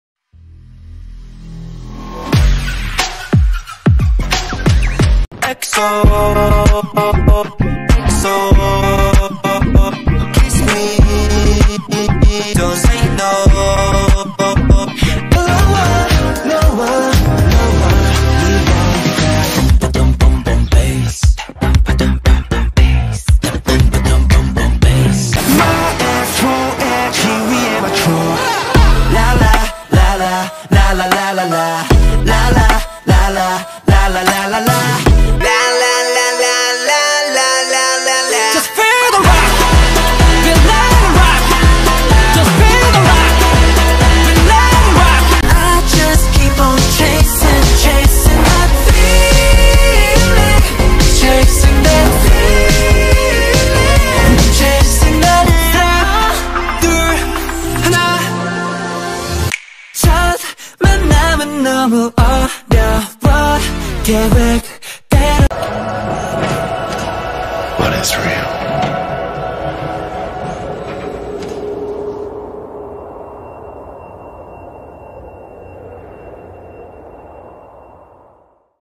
2024 Mnet MAMA Nomination Predictions - Best Dance Performance Male Group
NOTE: This are ONLY MY predictions about my selected category. Official nominees will be on 14th October at 5PM PHT (6PM KST)